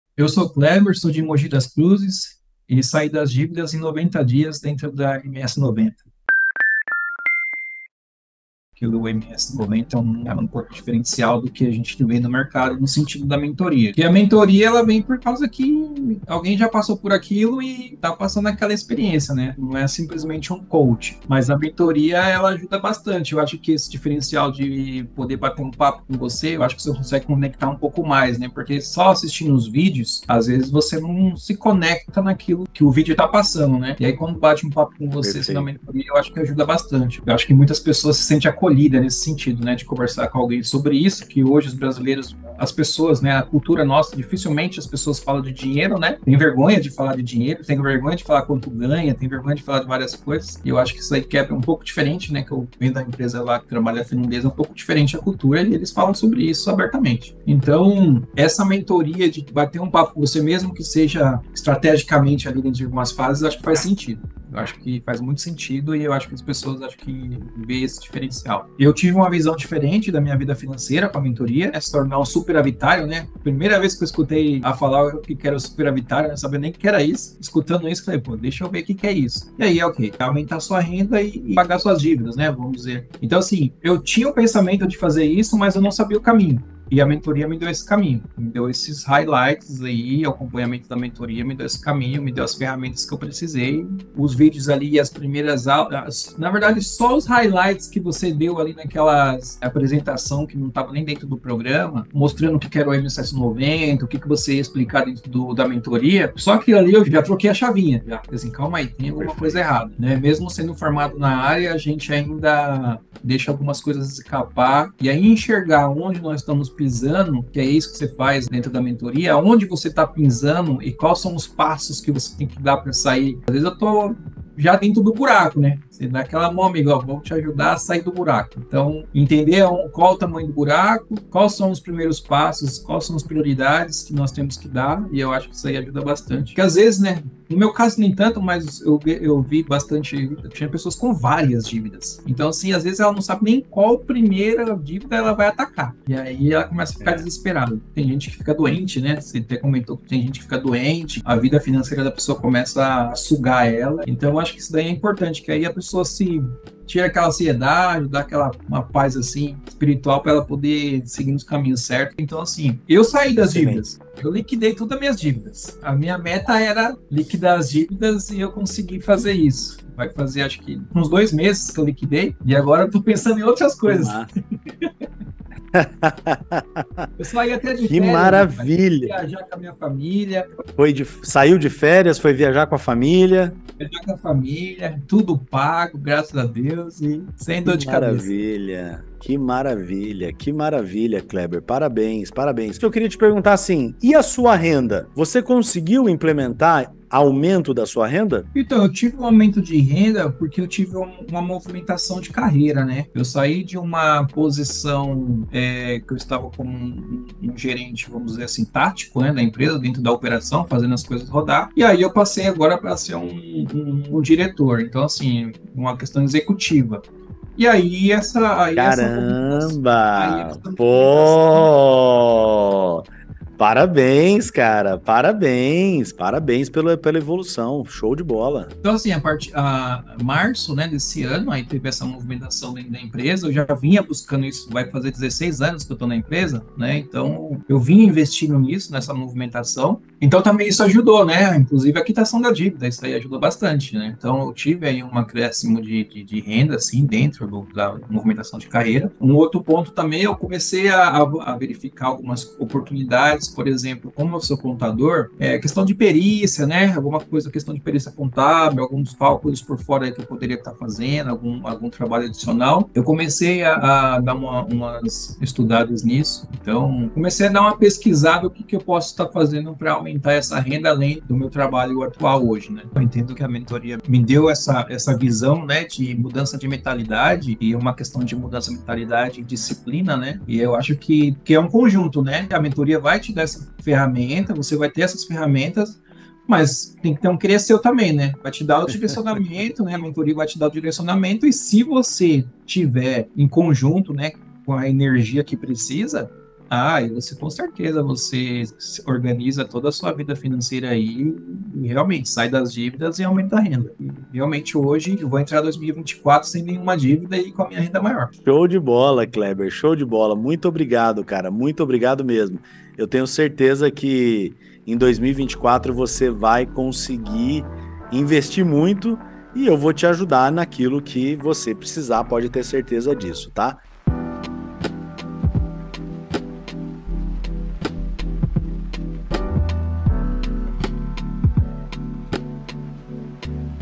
Cleber saiu das dívidas em 90 dais e começou 2024 com a renda maior
Neste vídeo, você vai conhecer Cleber, meu mentorando que sofria com as altas taxas e juros sobradas pelos bancos. Mas ele não se conformou com essa situação e resolveu tomar uma atitude, entrando para a MS90, a minha mentoria Superavitário em 90 dias. 😎✌